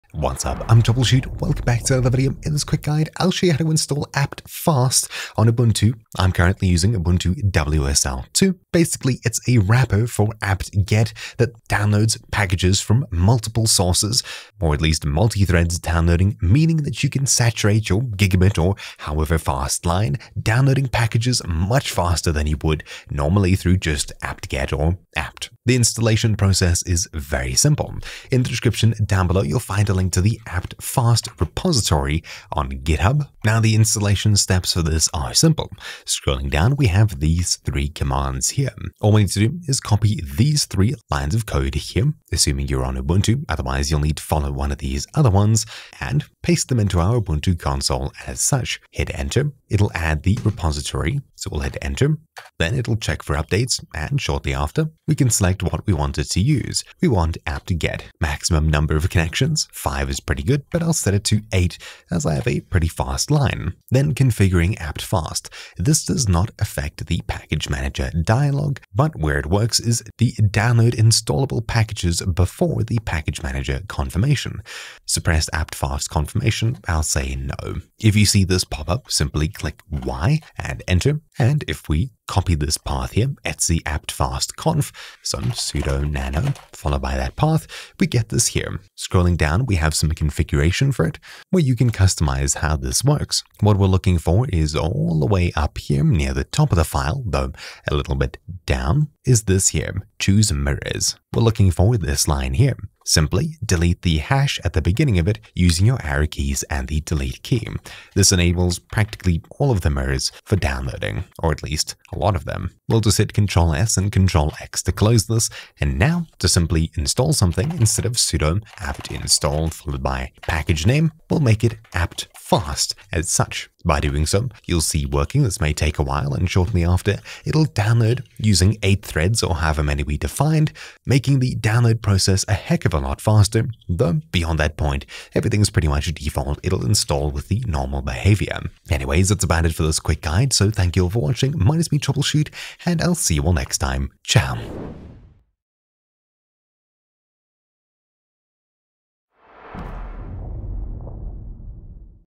Install Apt-Fast Guide | INSANELY Fast Software Installs & Download Speed
Looking to speed up your software installs and downloads? Check out this guide on how to install and use apt-fast, a powerful tool that can significantly improve your apt download times by allowing parallel downloads with multiple connections per package. Say goodbye to slow downloads and hello to lightning-fast software installations with apt-fast. Watch now to learn more!

Timestamps:
0:00 - Explanation & What is Apt-Fast
0:25 - Installing apt-fast
1:20 - Configuring apt-fast
1:55 - Installing with apt-fast